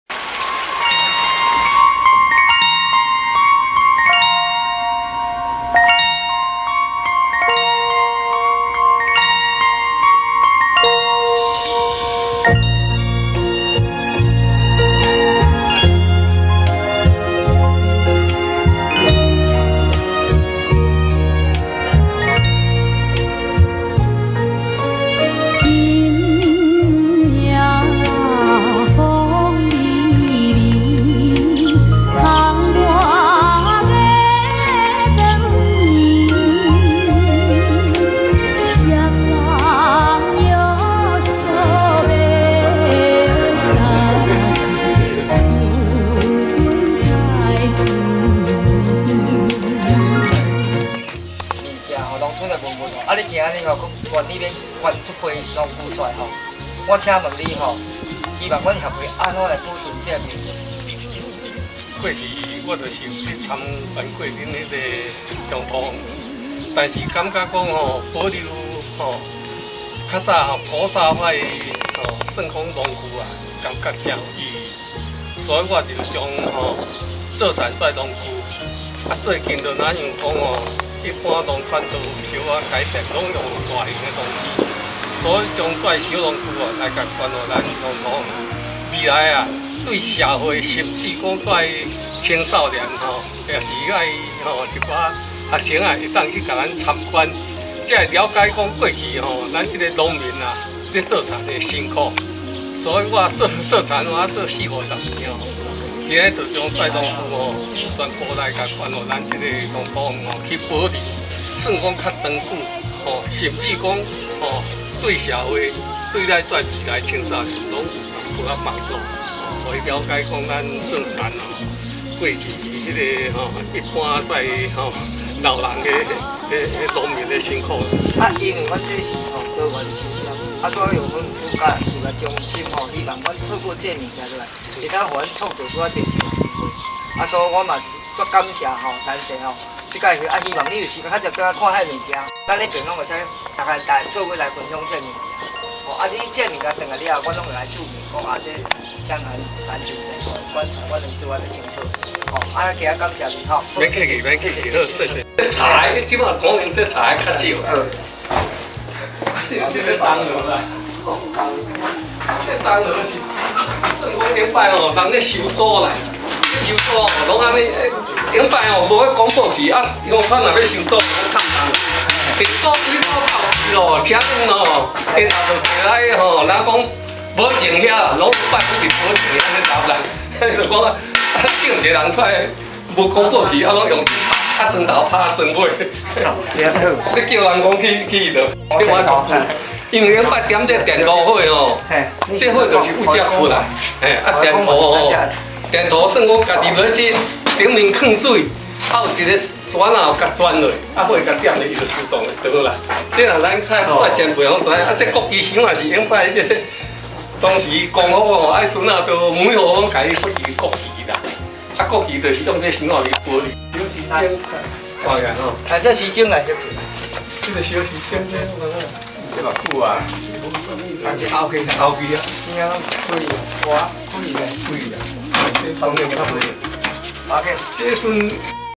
頂菜園-搶救農村資源再生-2
等待重返榮耀的農村文物，一個方案造就農村轉型，成為社區的願景，且結合社會資源、教學觀摩、社區參休閒旅遊等，讓多元就業到社區企業在荒村萌芽。